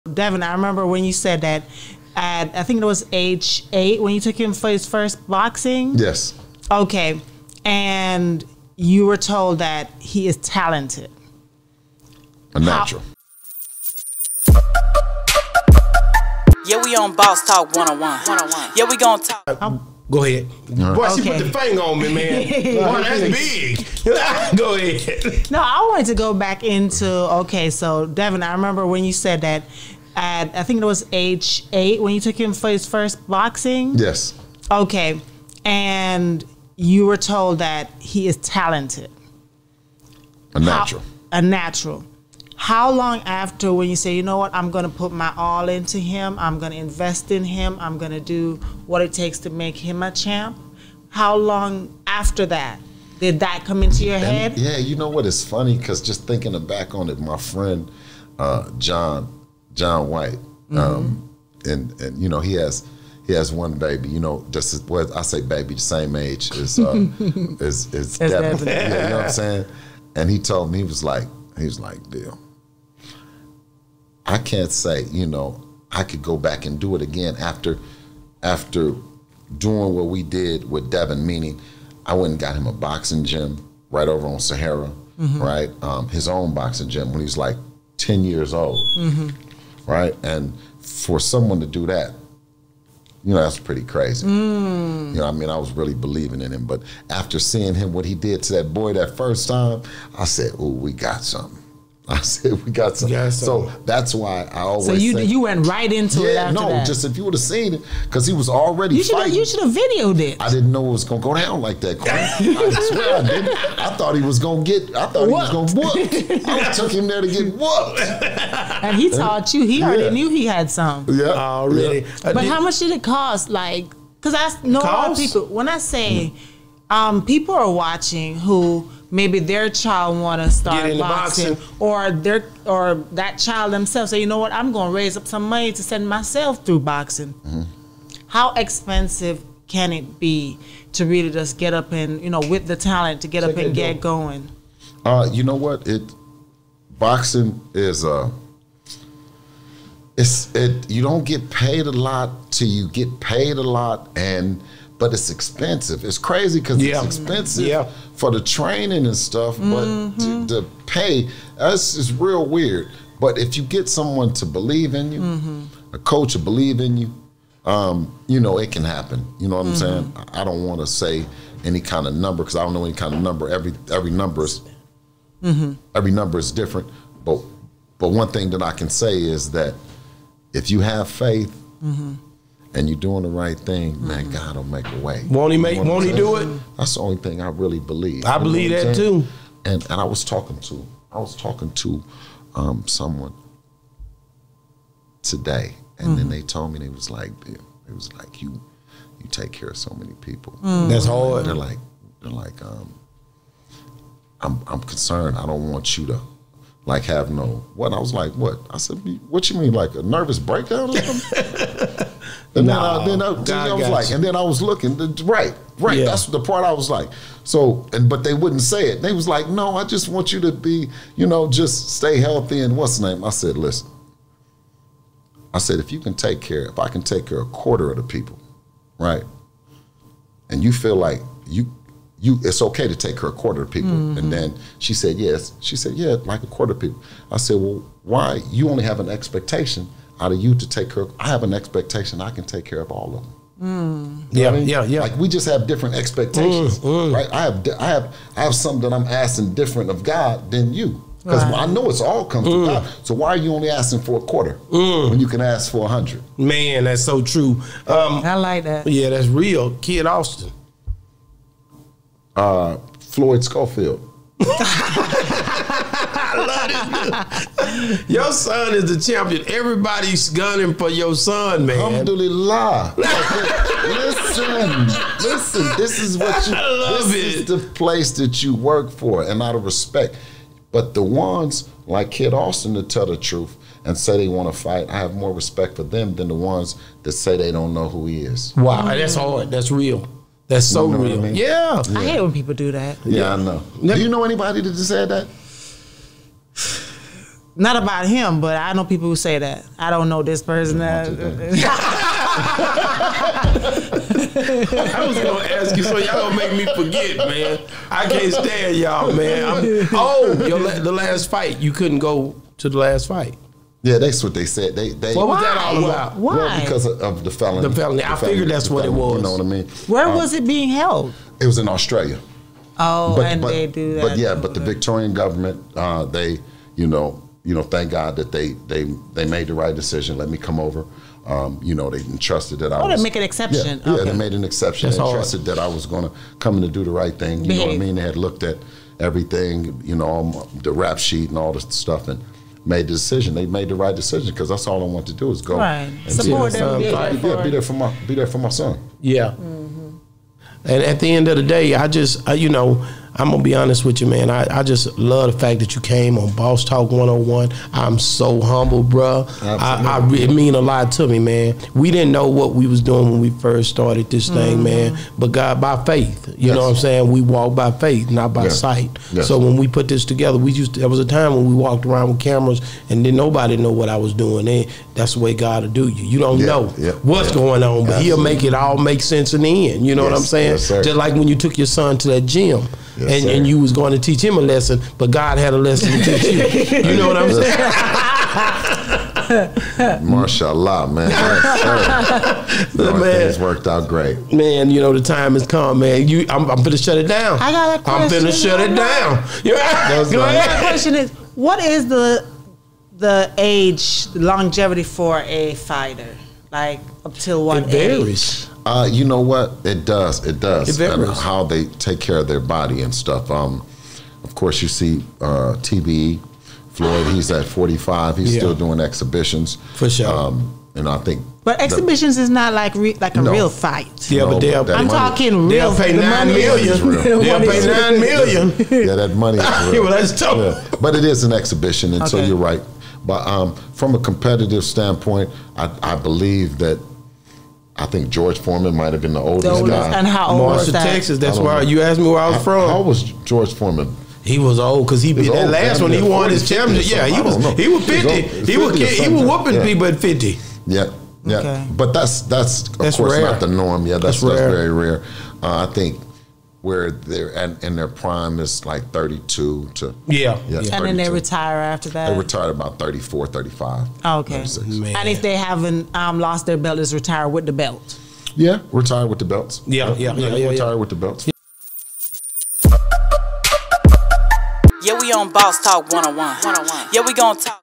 Bill Haney on The 1st Time Seeing Devin Haney Fight, Thoughts Kid Austin | George Foreman (Part 5)
Bill Haney speaks My Son is the Champ‼️

Bill Haney on The 1st Time Seeing Devin Haney Fight, Thoughts Kid Austin | George Foreman